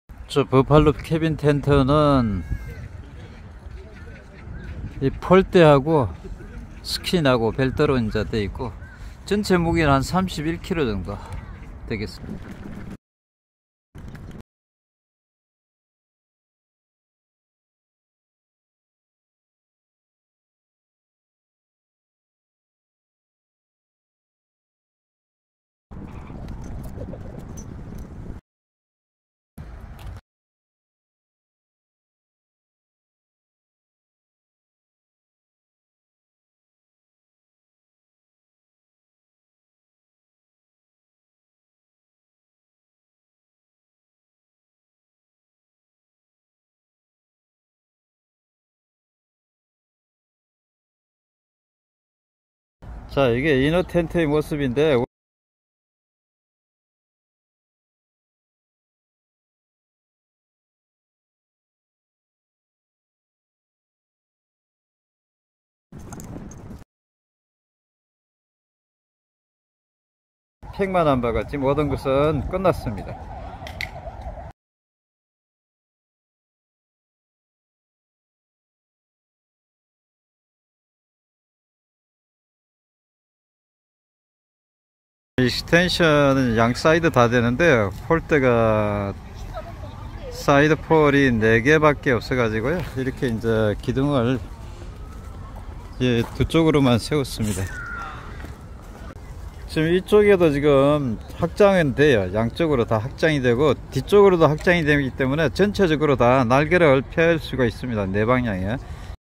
[왔다캠핑 Korea camping traveler - 캐빈텐트 피칭]버팔로 카사캐빈텐트 피칭 ~ 4배속,buffalo Kasakevin tent as speed pitching
버팔로 캐빈텐트 스피드 피칭